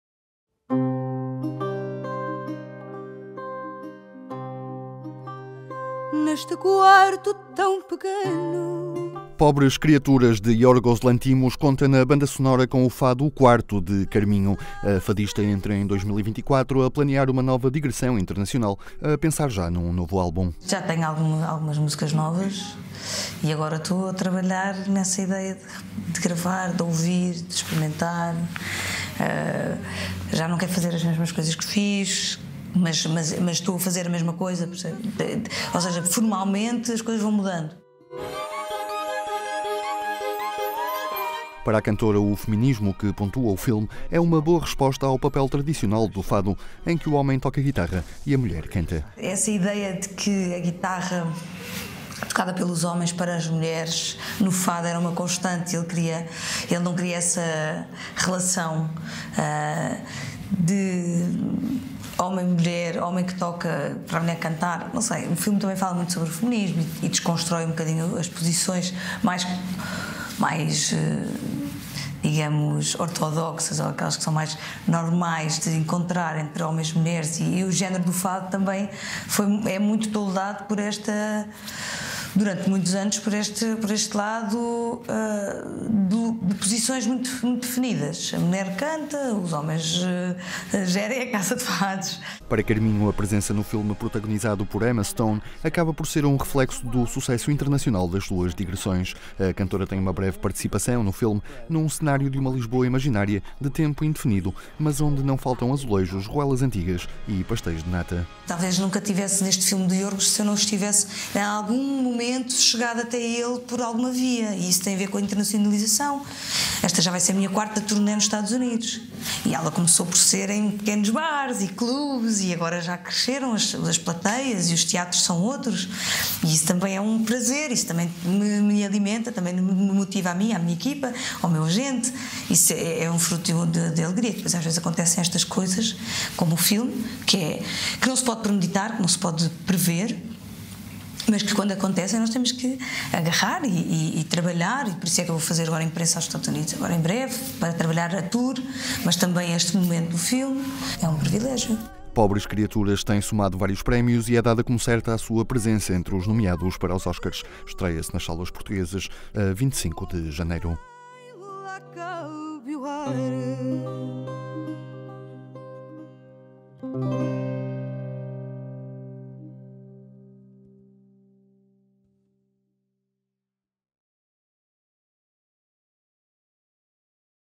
Para Carminho, 2024 vai ser "um ano rico" no cinema, em estúdio e na estrada
A fadista Carminho entra em 2024 a planear uma nova digressão internacional, a pensar já num novo álbum e a colher os frutos de experiências imprevistas, como entrar no filme “Pobres Criaturas”, de Yorgos Lanthimos.